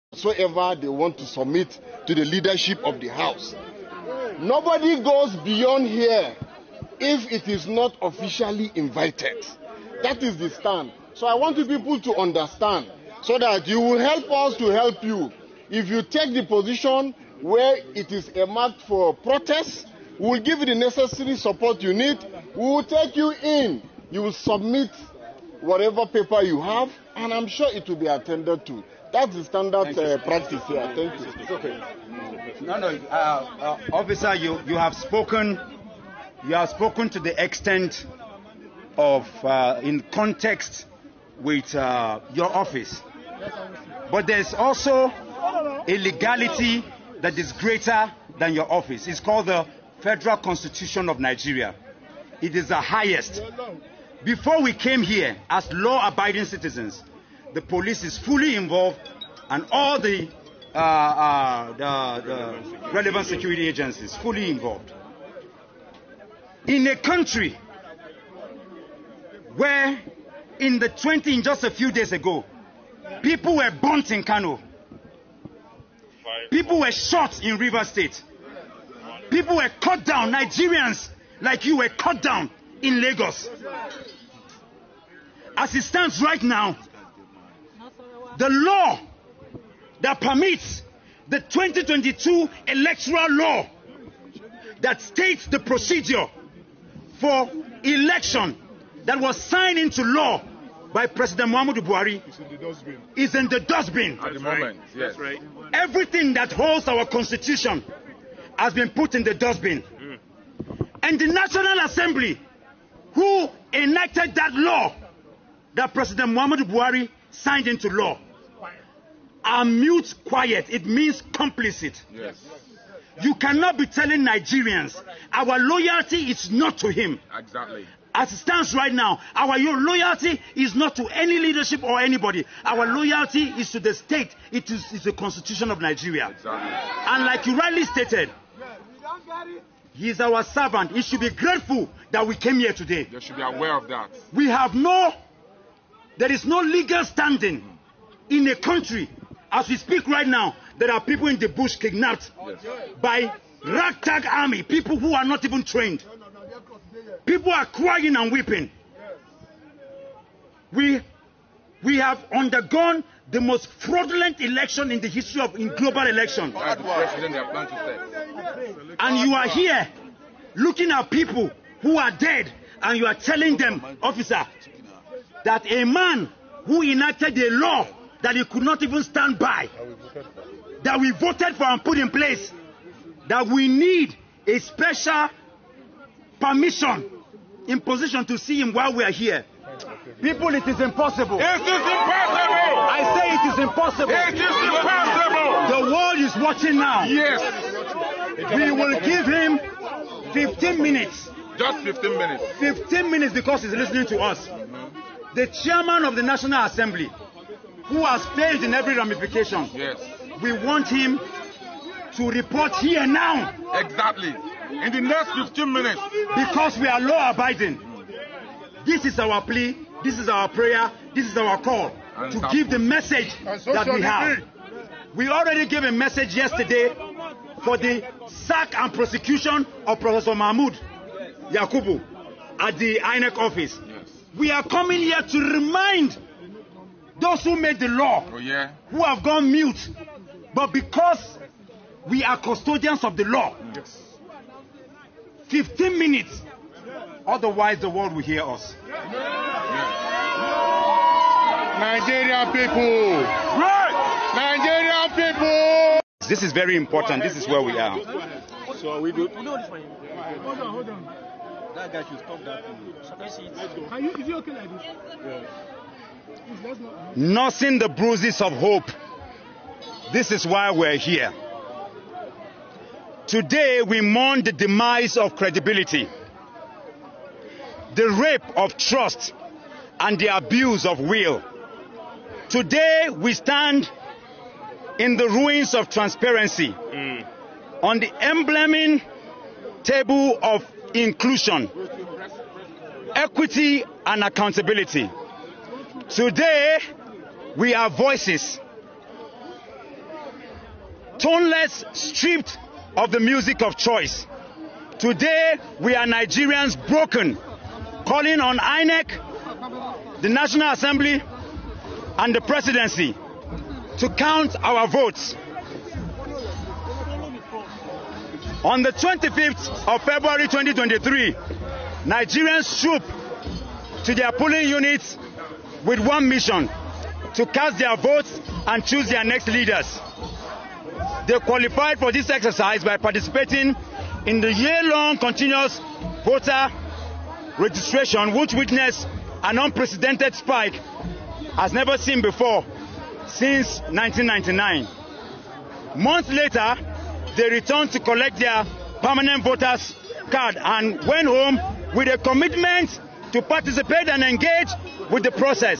BREAKING:
As rightly planned, protest has started from Abuja day 3, and is going to spread to other regions except Lagos for reason of not escalating Igbo-Yoruba fight but later Lagos will joinLogistics of transportation, food n drinks have been put in place